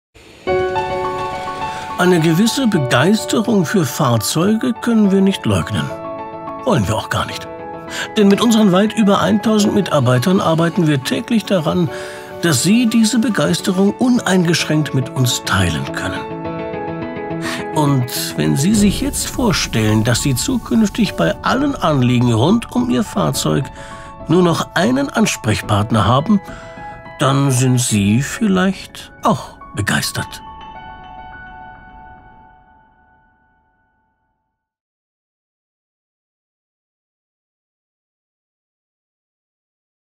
IDENTICA Imagefilm Lehmann (HD)
Der offizielle IDENTICA-Imagefilm der Autolackierung Lehmann GmbH.